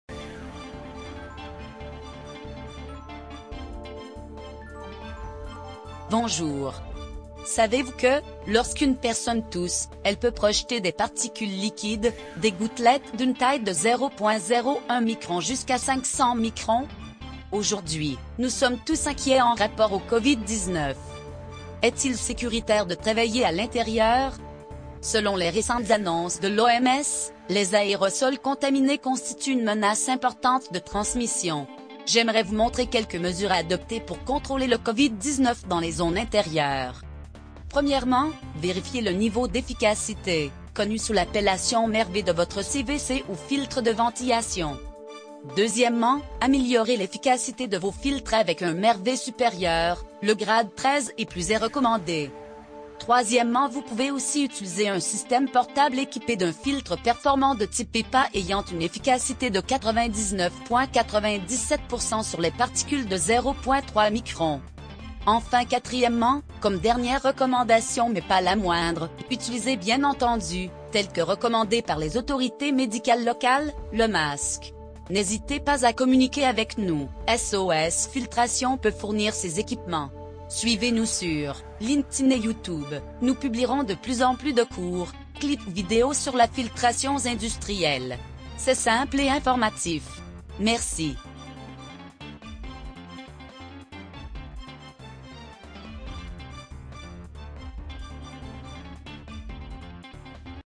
4 Étapes pour éliminer COVID 19
Bonjour, savez-vous que lorsqu'une personne tousse, elle peut  projeter des particules liquides, des gouttelettes d’une taille de 0,01 micron jusqu’à 500 microns? Aujourd’hui, nous sommes tous inquiets en rapport au COVID-19. Est-il sécuritaire de travailler à l’intérieur?

Selon les récentes annonces de l’OMS, les aérosols contaminés constituent une menace importante de transmission. J'aimerais vous montrer quelques mesures à adopter pour contrôler le COVID 19 dans les zones intérieures. Premièrement, vérifiez le niveau d’efficacité, connu sous l’appellation MERV de votre CVC ou filtre de ventilation. Deuxièmement, améliorer l’efficacité de vos filtres avec un MERV supérieur, le grade 13 et plus est recommandé. Troisièmement vous pouvez aussi utiliser un système portable équipé d’un filtre performant de type HEPA ayant une efficacité de 99.97% sur les particules de 0.3 micron.

Enfin quatrièmement, comme  dernière  recommandation mais pas la moindre, utilisez bien entendu, tel que recommandé par les autorités médicales locales, le masque. N’hésitez pas à communiquer avec nous, SOS Filtration peut fournir ces équipements. Merci!